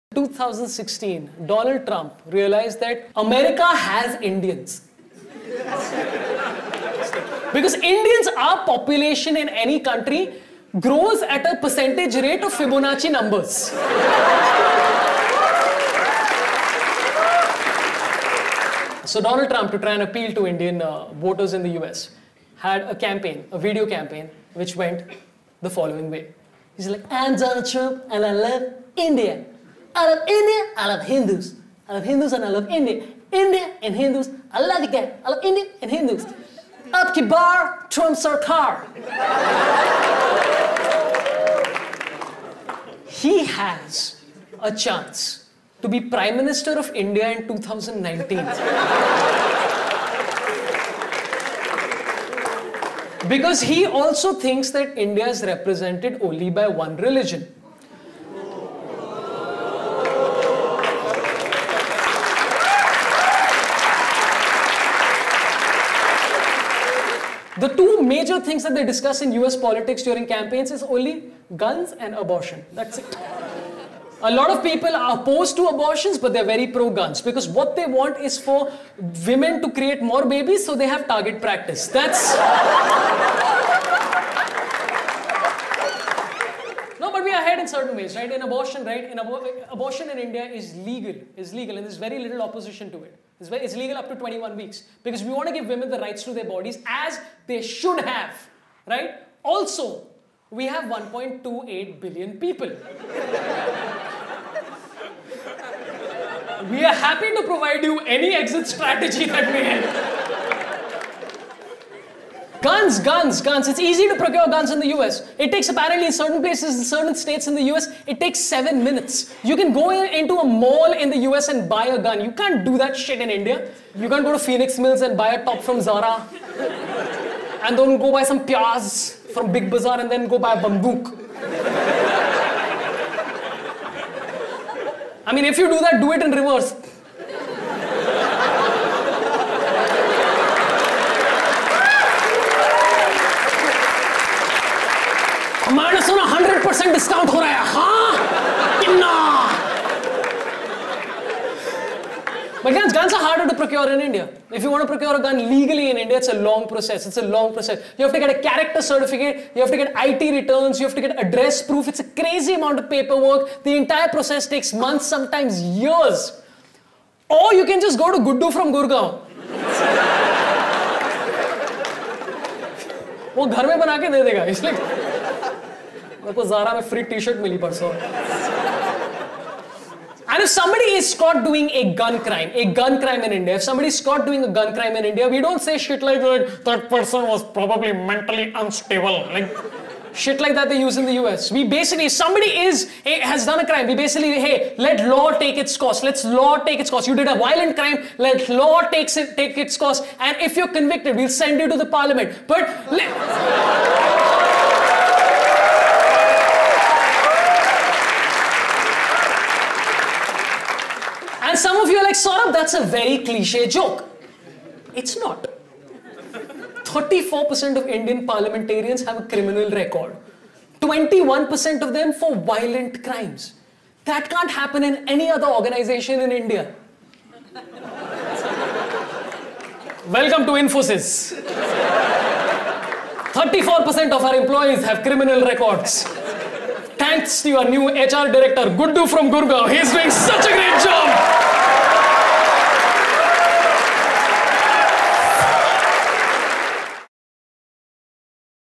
Guns in America u0026 India Standup Comedy by Sorabh Pant
We keep comparing ourselves to the U.S.
But India *trumps* America in gun laws. Kind of.